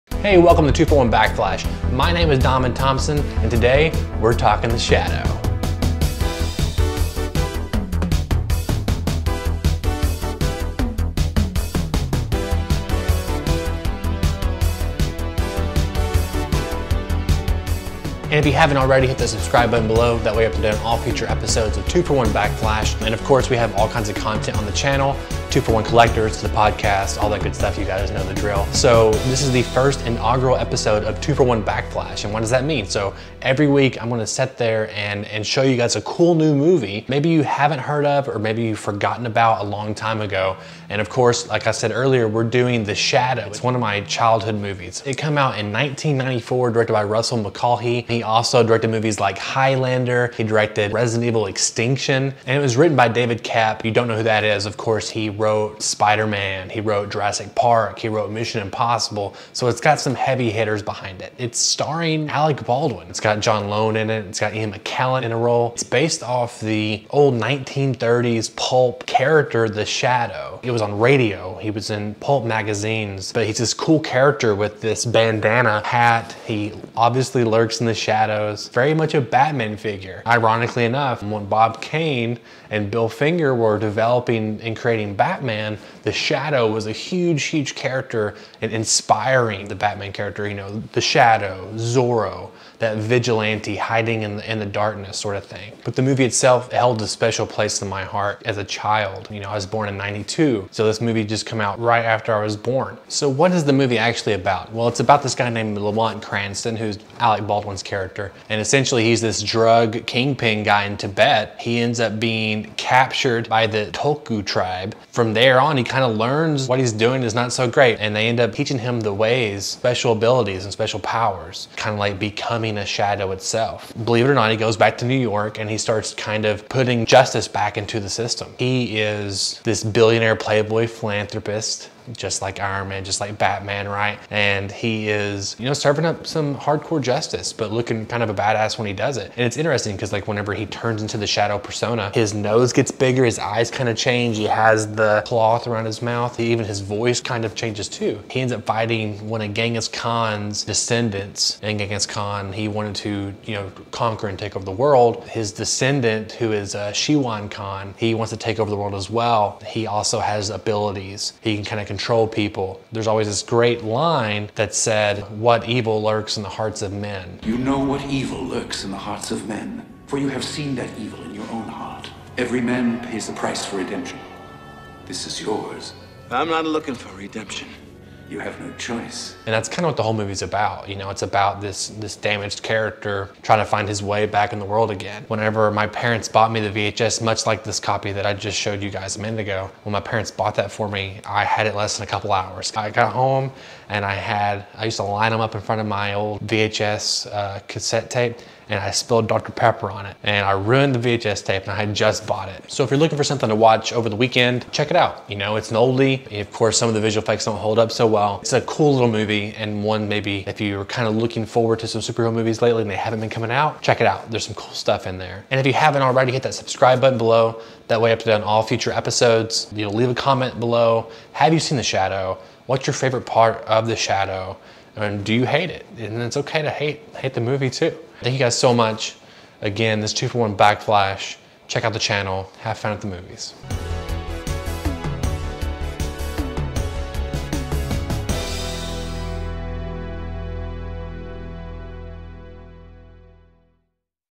THE SHADOW presented by 241 Backflash
Welcome to 241 Backflash! The show where Donavon Thompson from 241 Studios presents a movie from the past! Maybe you've seen it, maybe you haven't, but Donavon puts the cinema spotlight this week on, "The Shadow" (1994). Directed by Russell Mulcahy and starring Alec Baldwin, "The Shadow" is a fun movie and deserves some much needed attention. What evil lurks in the hearts of men?!

And sound off in the comments below! Have YOU seen "The Shadow"? What did you think? And what movies have you seen that you love but pretty sure no one has heard of before? Let me know so I can watch it!

Have comments or questions? Click the link below and leave us a message!